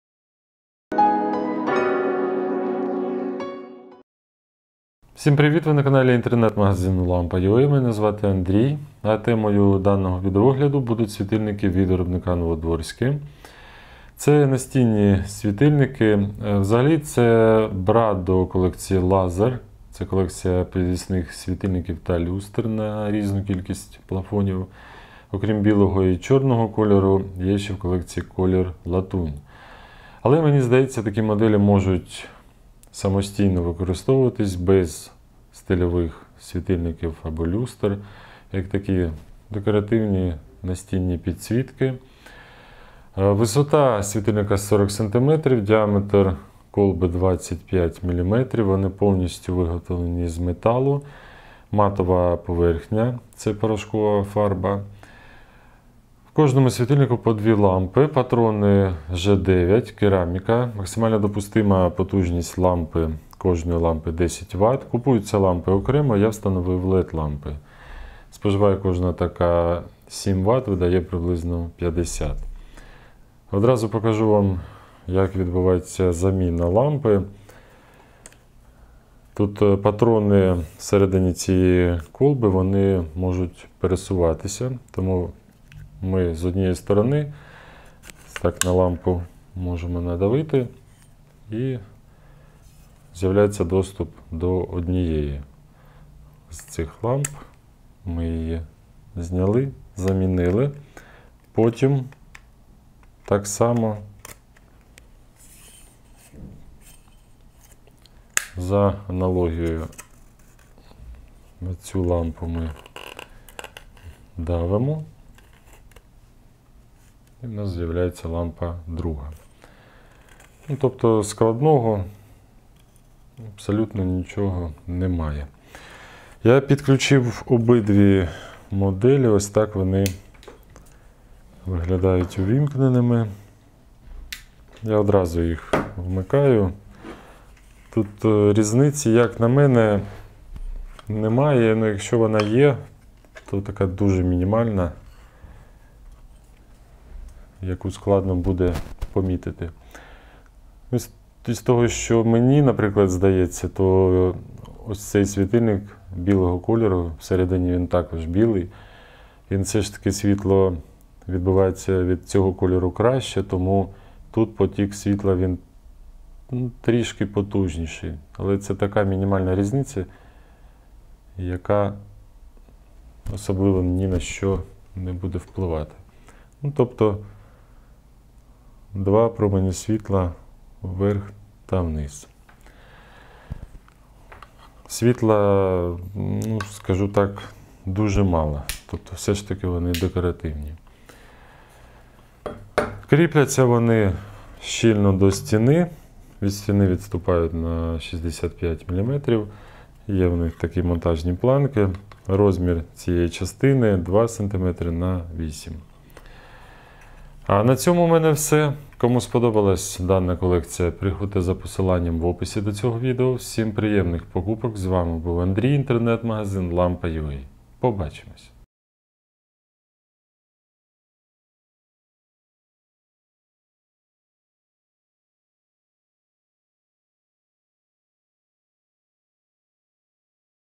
Бра NOWODVORSKI 49694, 49695 огляд
Купити біле і чорне бра NOWODVORSKI 49694, 49695 (NOWODVORSKI 8048, 8047 Laser)

Огляд білого і чорного світлодіодного бра в стилі лофт і модерн з колекції Laser від бренду NOWODVORSKI (Польща)

  З ролика ви дізнаєтесь про:
  - інші світильники з колекції;
  - тип патрона;
  - матеріали світильника;
  - технічні характеристики;
  - максимальна потужність використання світильника;
  - розсіювання освітлення;
  - діаметр креплення;